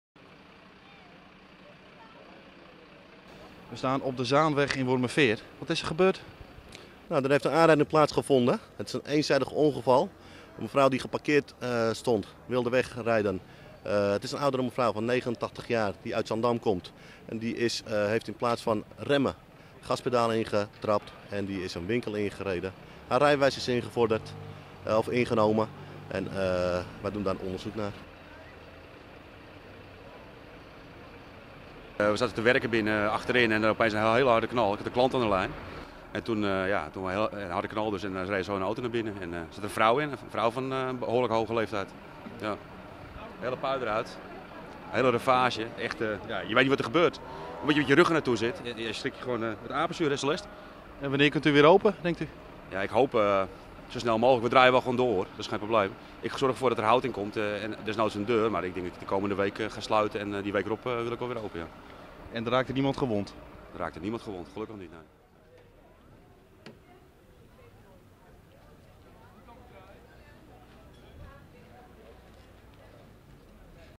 19-03-2011 Wormerveer - ongeval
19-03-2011 Wormerveer - Zaterdagochtend omstreeks elf uur is op de Zaanweg in Wormerveer een auto een winkel binnengereden.
 De bestuurder raakte lichtgewond.

 De 89-jarige vrouw reed de winkel Kozaan binnen toen zij per ongeluk het 'gas' intrapte in plaats van de rem.

 Het rijbewijs is ingenomen door de politie.
 De eigenaar van de zaan probeert volgende week weer helemaal open te zijn voor het publiek.